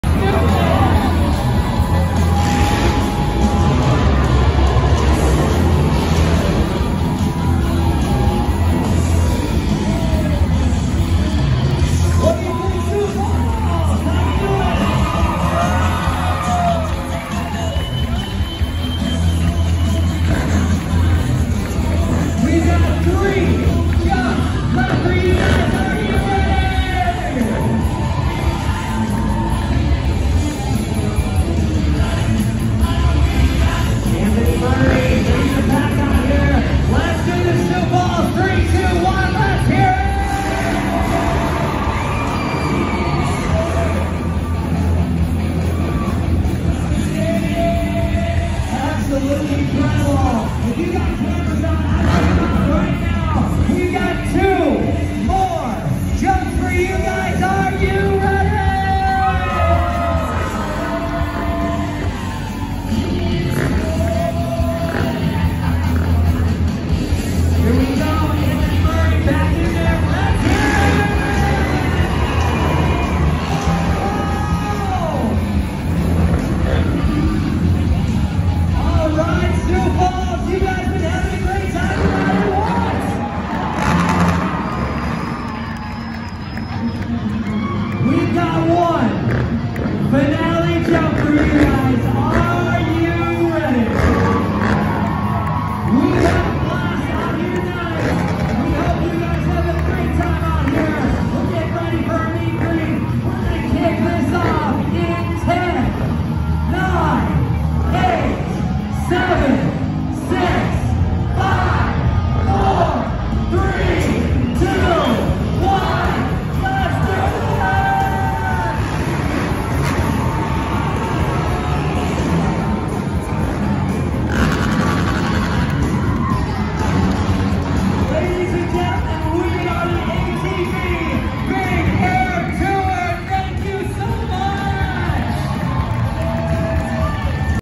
Last night Under The Hood with ATV Big Air Tour Massive High ATV Jumps Powerful ATV Engines
Huge jumps at Sioux Falls ATV Big Air Tour.